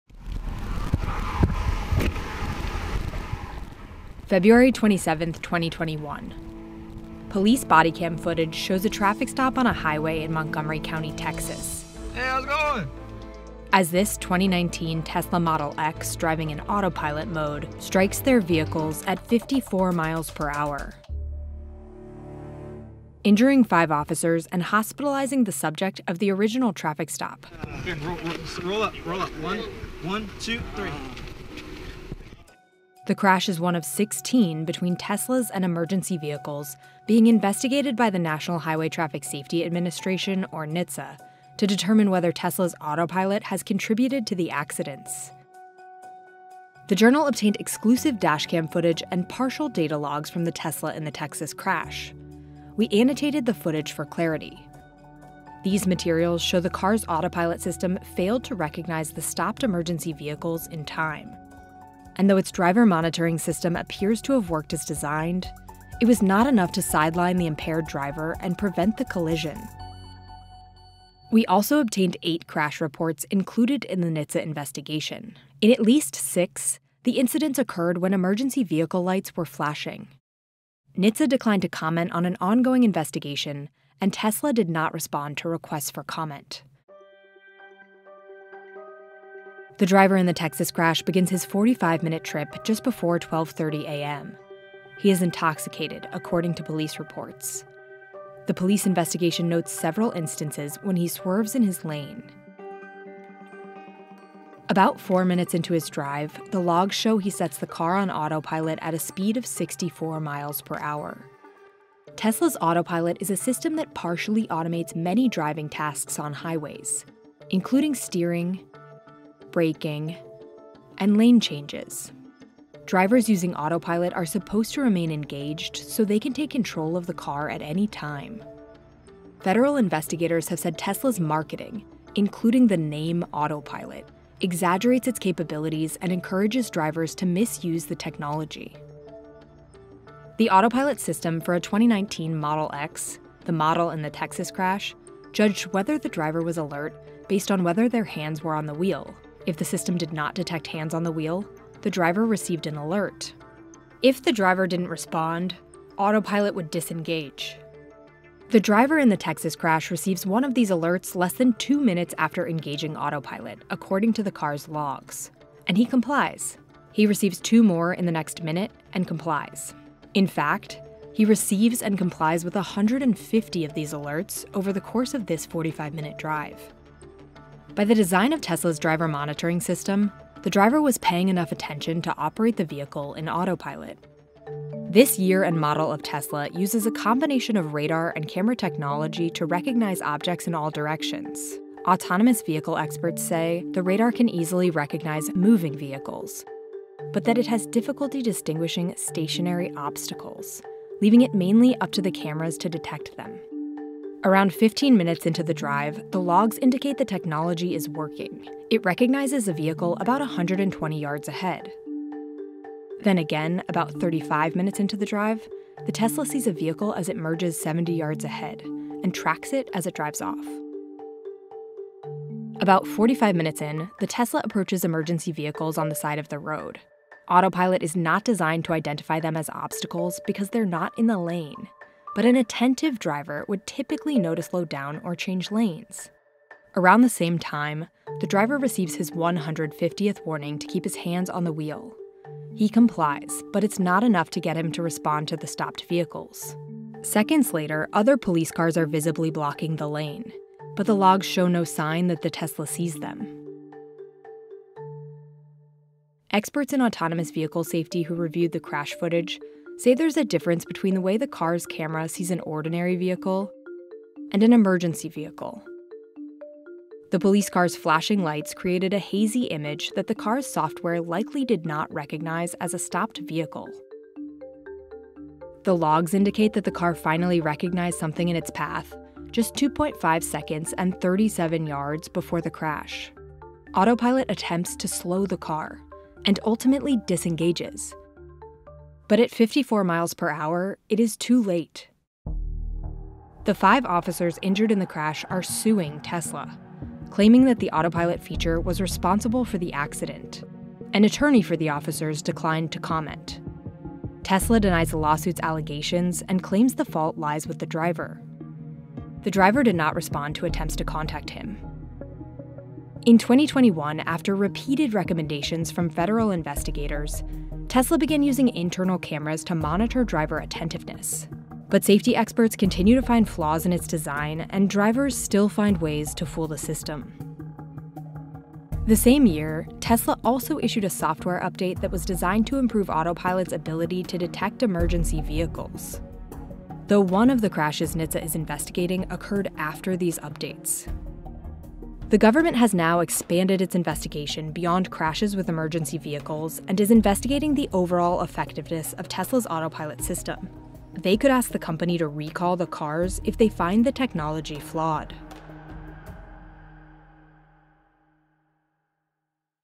Tesla Dashcam Footage Suggests Reasons for Autopilot Crashes | WSJ Exclusive
The federal government is investigating 16 crashes involving Teslas operating in autopilot mode and emergency vehicles. The Journal obtained exclusive dashcam footage from a Model X in one of these accidents.

In this video investigation, WSJ breaks down what happened and looks at the potential problems with Tesla’s autopilot technology.

0:00 Exclusive Tesla crash footage
0:32 A pattern of Teslas crashing into emergency vehicles
1:52 Tesla’s autopilot system
2:20 Autopilot in action - breaking down the drive
3:42 What went wrong? Breaking down the crash
4:52 Lawsuits and investigations against Tesla

WSJ video investigations use visual evidence to reveal the truth behind the most important stories of the day.